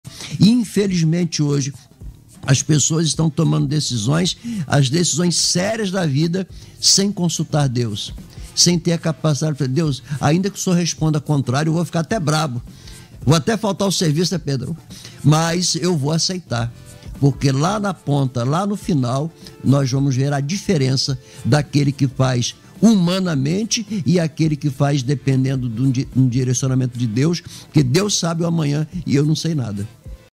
Debate Melodia - Sempre consulte Deus!
Debate Melodia: temas polêmicos analisados à luz da Bíblia. Um verdadeiro seminário no ar!
Tema:  Certo ou Errado: As CONSEQUÊNCIAS DE UMA ESCOLHA